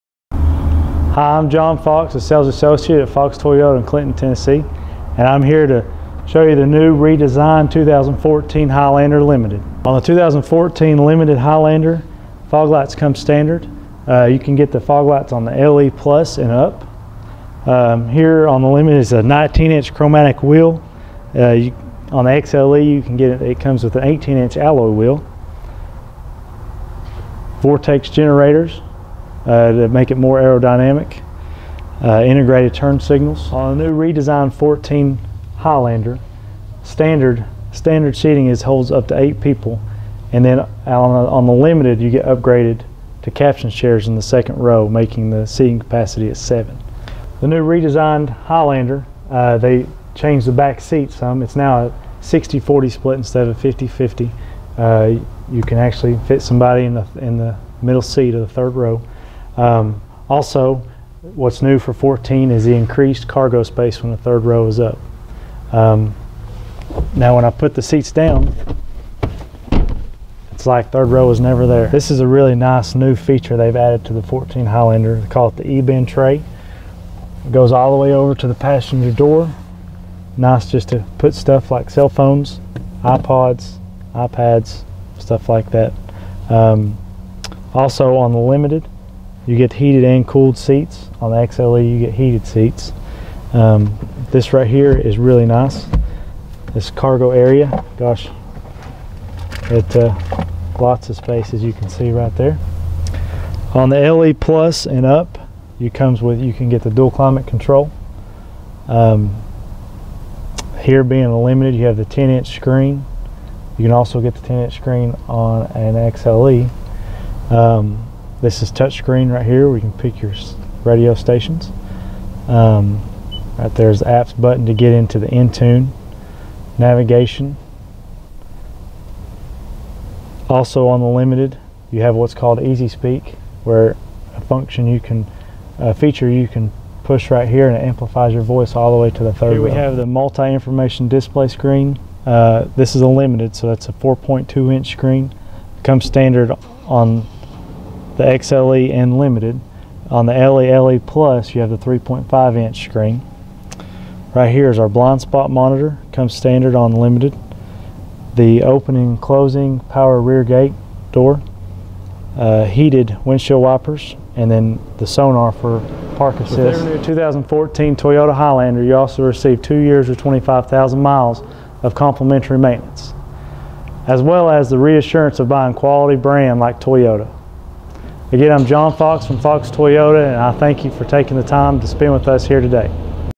2014 Highlander Walk-around - John Fox - Fox Toyota
John Fox gives a brief walk-around of the 2014 Highlander